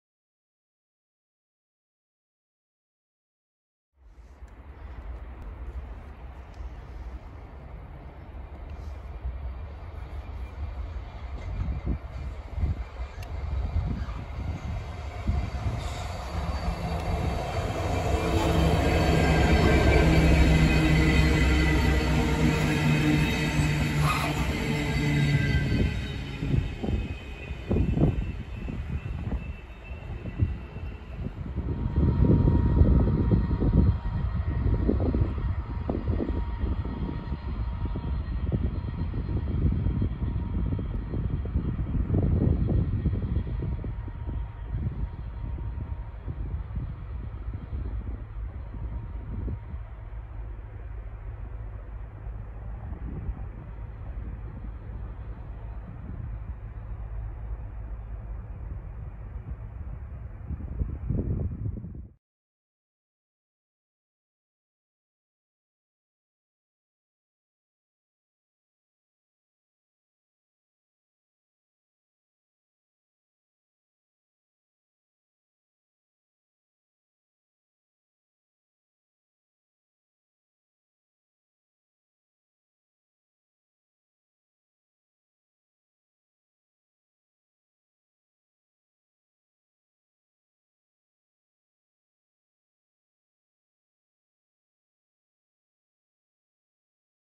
66109, 66165 & 66130 at Darlington - 0K01 Millerhill S.S. to Tees N.Y.
66109 - ‘Teesport Express’
66165
66130